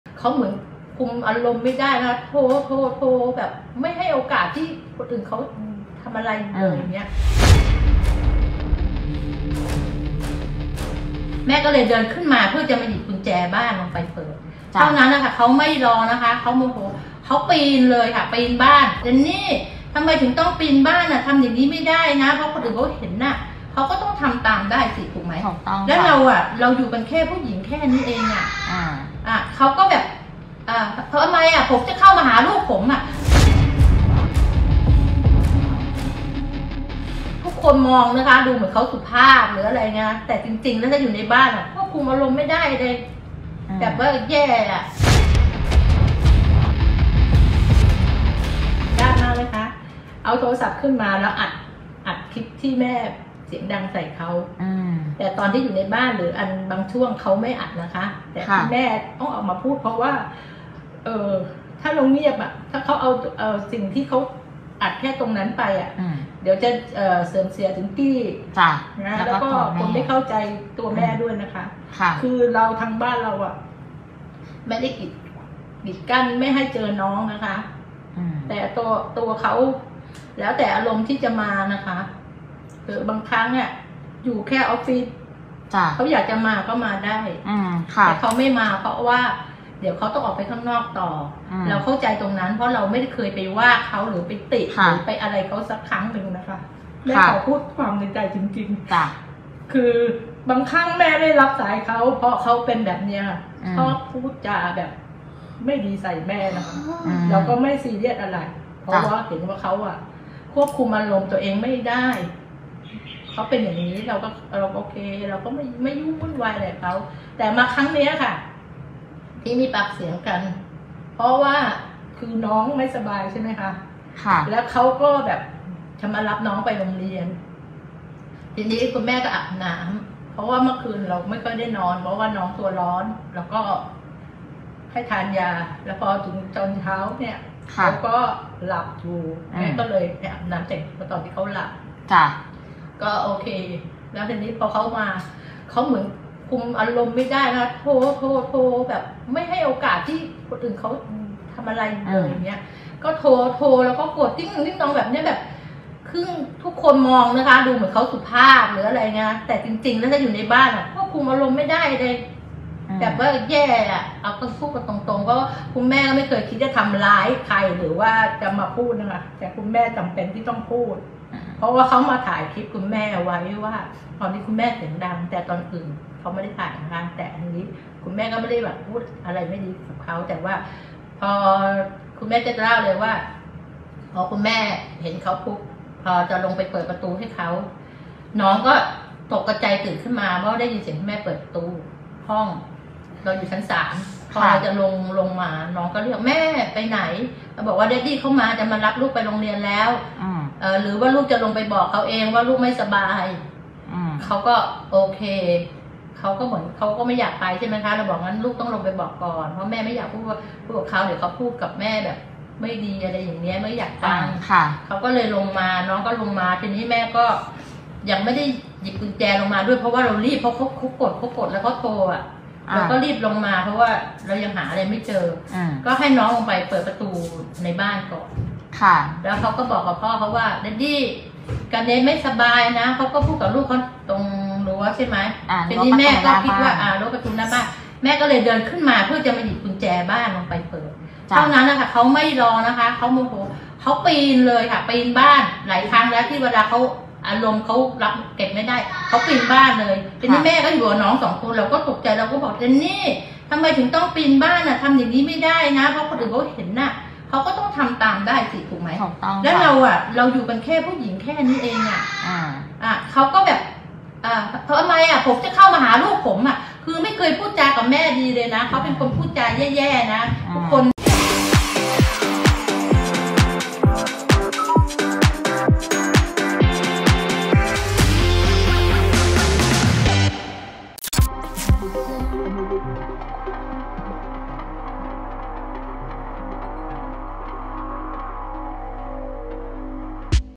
คนสุภาพแค่ฉากหน้า แม่มิ้วกี้ผวา “แดนนี่” ปีนเข้าบ้าน | คลิปเดียวรู้เรื่อง
แม่มิ้วกี้ไลฟ์เล่านาทีผวา “แดนนี่” เลือดร้อนปีนบ้านอดีตเมีย แฉคนสุภาพแค่ฉากหน้า